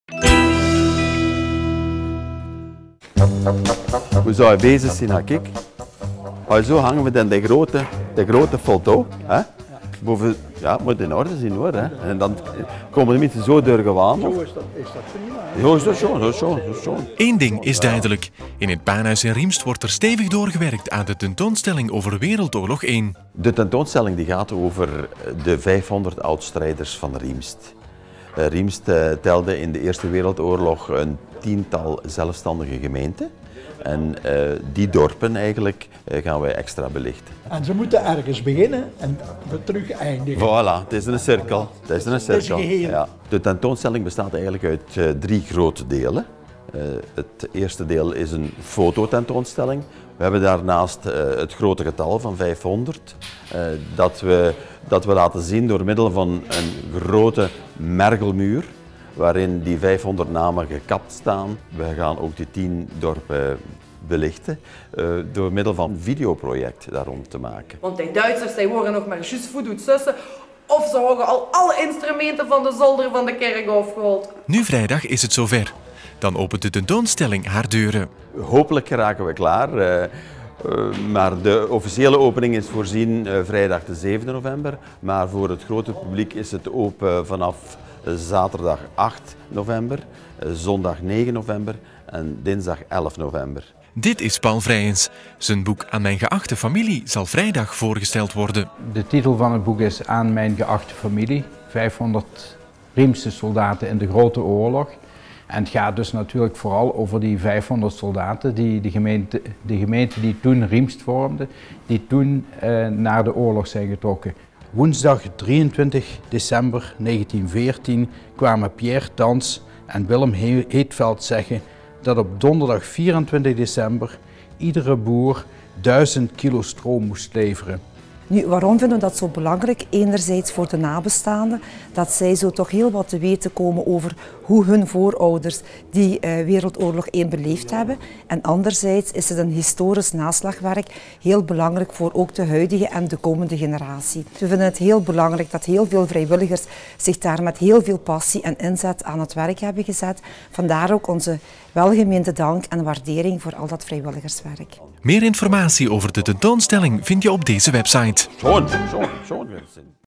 Riemst: De dorpen van Riemst in 14-18
Tentoonstelling in zaal 't Paenhuys over WO I. Muziekstuk: traditionele oorlogsliederen WO I. Aan de hand van videobeelden worden 10 monologen getoond. In de 10 dialecten van de Riemstse kerkdorpen worden verhalen verteld.Kunstwerken van lokale kunstenaars worden tentoongesteld. Voorstelling nieuwe publicatie Riemstse oudstrijders.